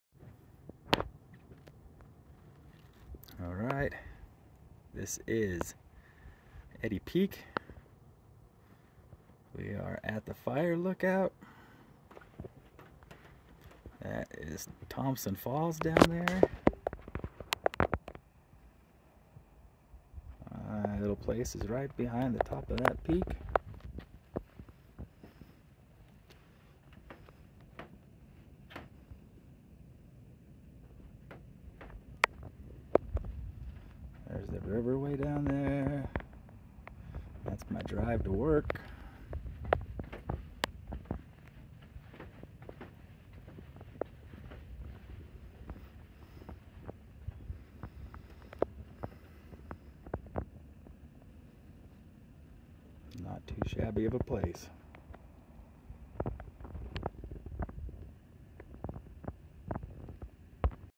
Eddy Peak Fire Lookout
Fire lookout between Thompson Falls and Plains Montana.